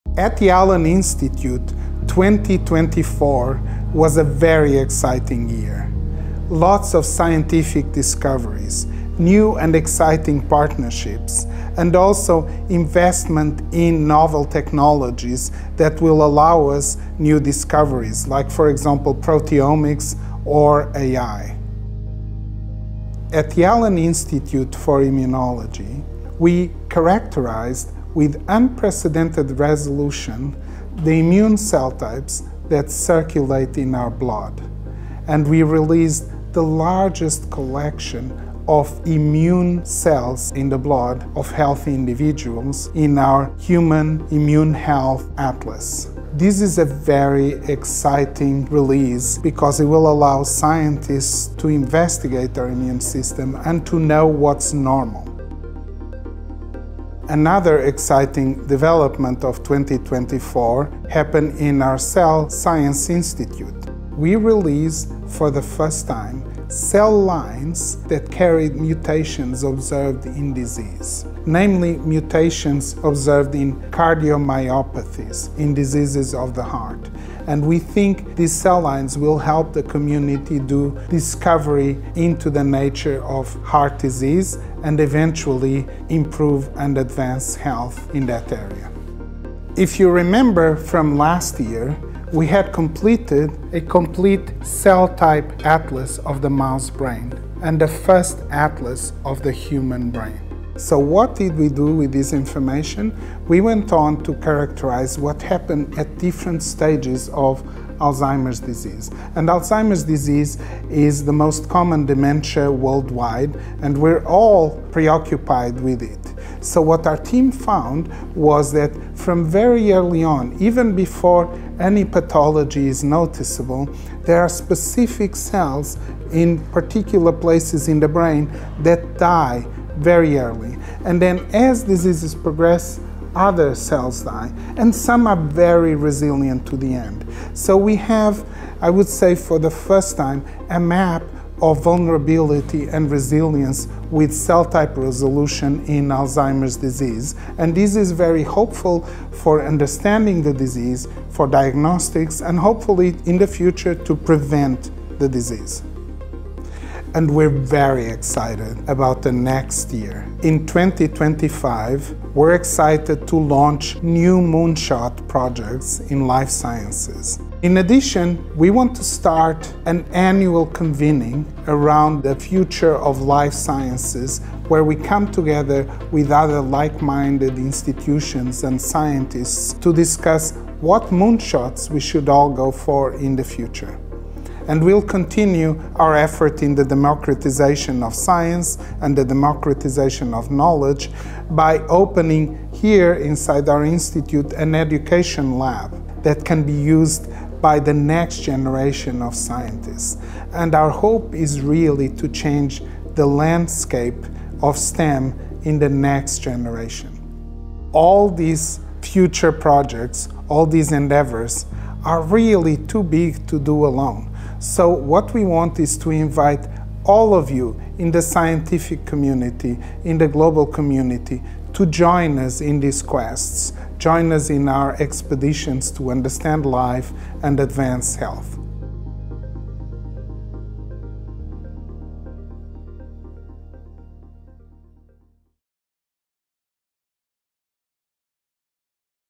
2024 – A year of breakthroughs and collaboration at the Allen Institute
President and CEO, Rui Costa, D.V.M., Ph.D., reflects on a transformative year for the Allen Institute. From unveiling the Human Immune Health Atlas to pioneering new cell lines to advance equitable disease research, to new findings related to Alzheimer’s disease, a commitment to open science, and collaboration has driven advancements in understanding life and advancing health.